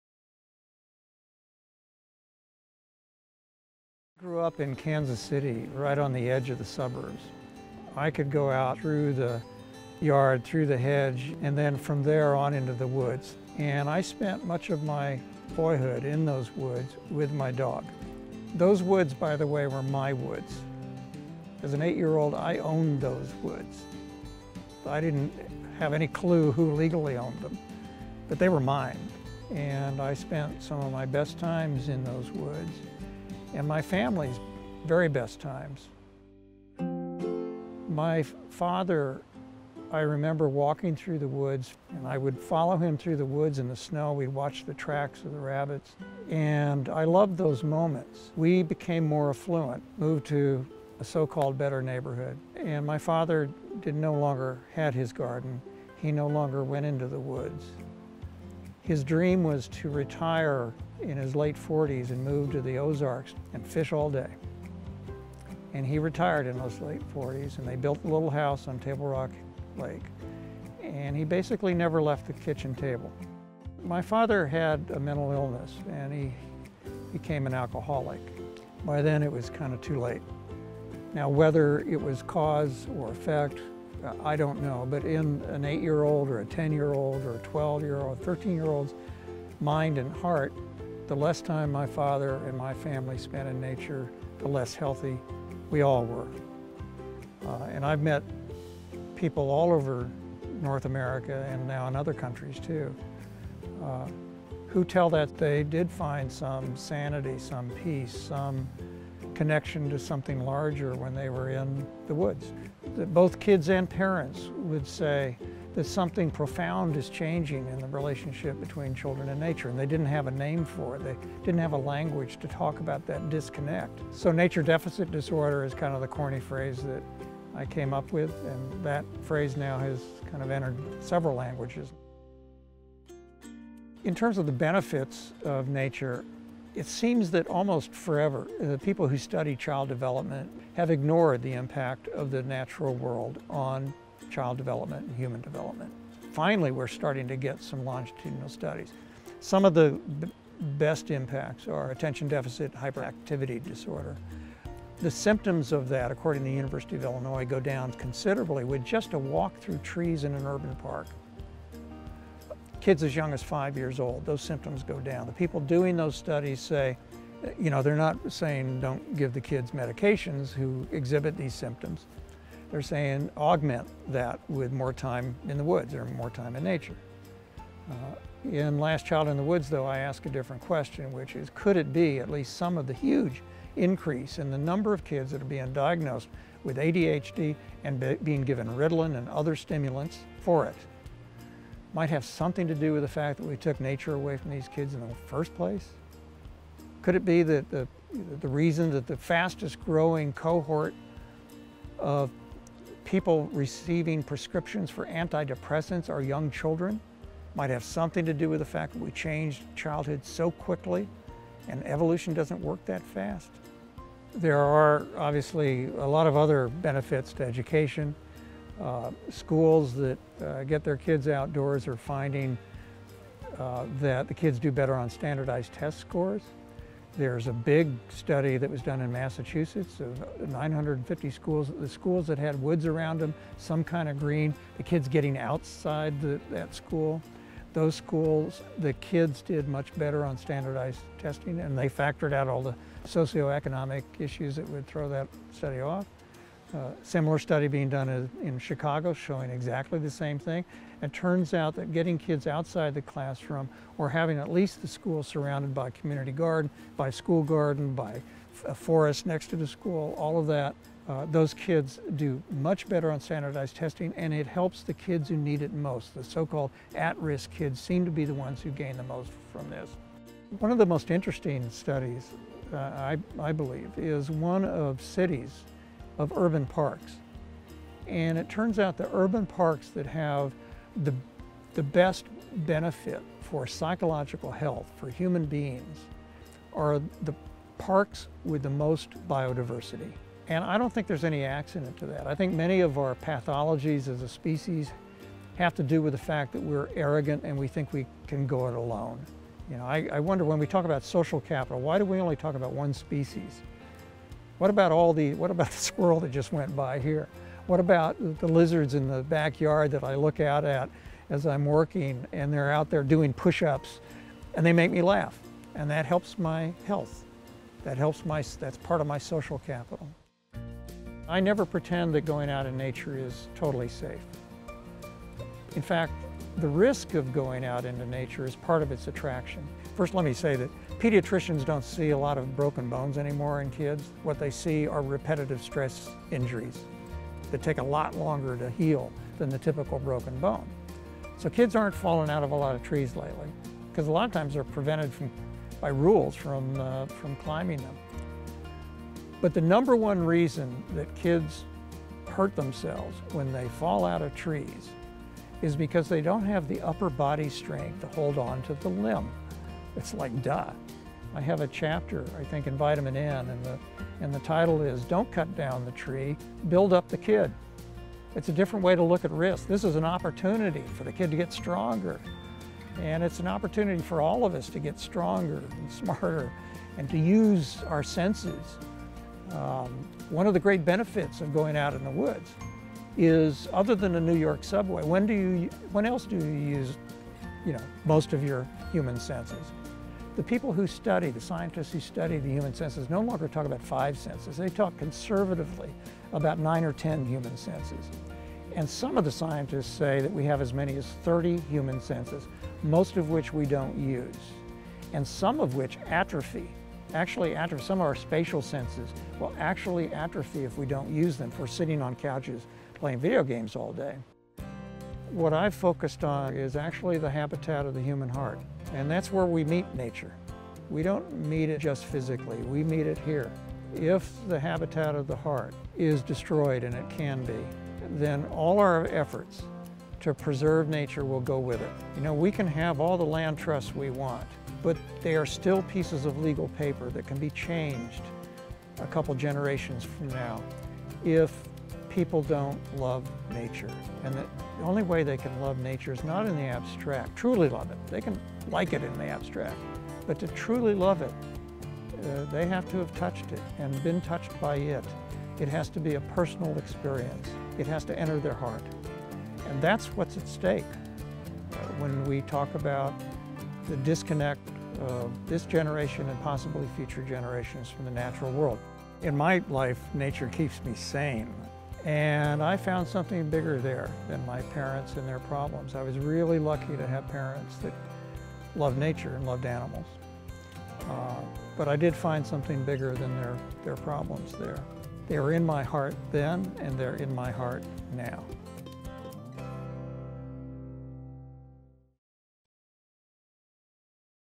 Why Forests Matter 2016: Richard Louv
The Why Forests Matter speaker series is a thought-provoking evening of discussion as a panel of experts expand the views of Canada’s forests as a resource, an inspiration and a place that sustains life.

Richard Louv - Author of Las Child in the Woods - shares his perspective on the value of forests in our lives.